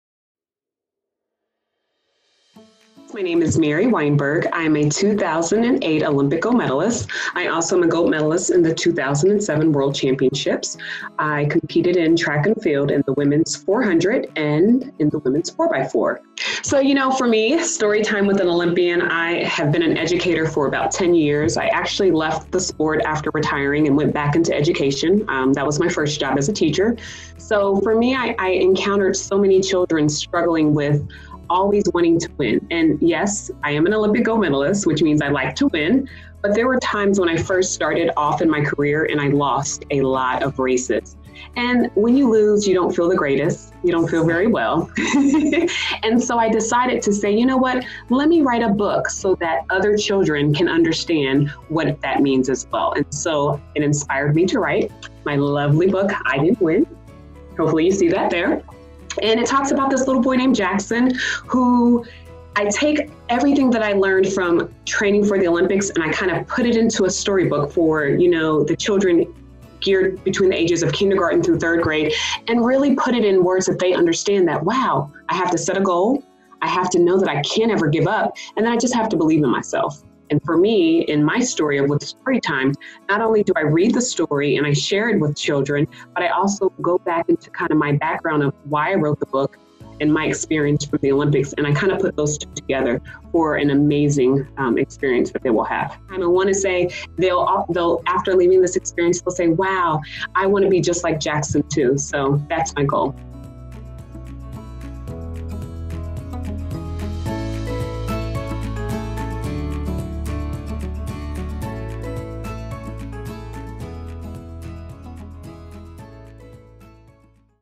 How Olympic gold medallist Mary Wineberg is inspiring children around the world
Olympic gold medallist Mary Wineberg is sharing her inspirational children’s book with young fans all over the world through the Olympian & Paralympian Online Experiences available on Worldwide Olympic Partner Airbnb’s platform.